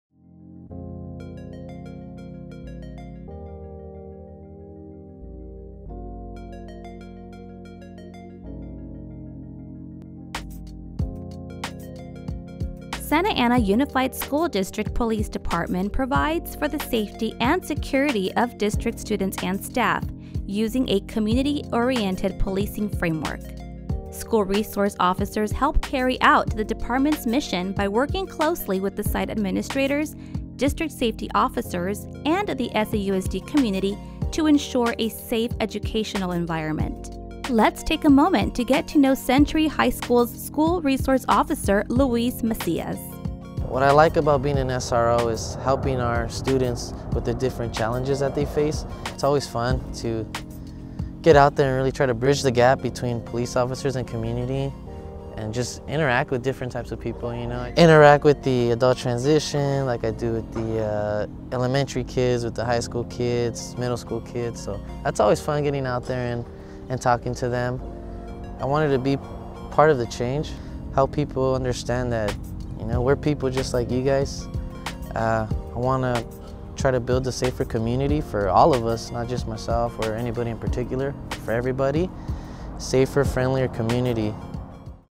Meet Your SROs - Century High School - Luis Macias
Santa Ana Unified School District Police Department provides for the safety and security of District students and staff, using a Community Oriented Policing framework.

School Resource Officers help carry out the Department’s mission by working closely with site administrators, District Safety Officers, and the SAUSD community, to ensure a safe educational environment.

Let’s take a moment to get to know Century High School’s School Resource Officer, Luis Macias